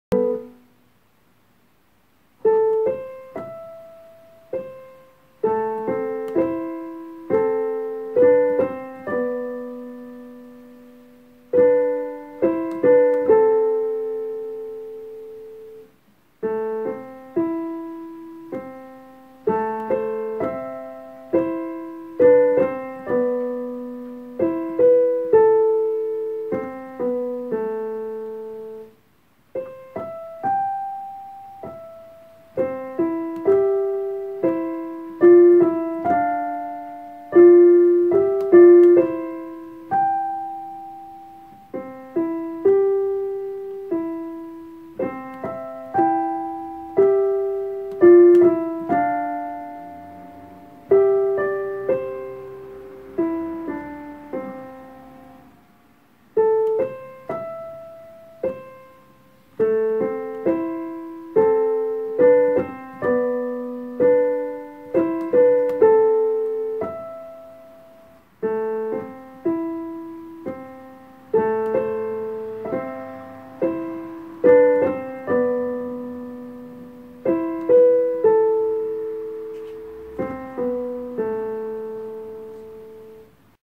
Piyano Çalıyorum
Piyano dersleri ilk eserim. Yorumlarda çok acımasız olmayın lütfen belki bir gün büyük bir piyanist olurum.
Neden olmasın ... Hayal etmesi bile güzel..  Eserin adını hatırlamıyorum malesef, hatırlayanlar lütfen yorum olarak yazsınlar..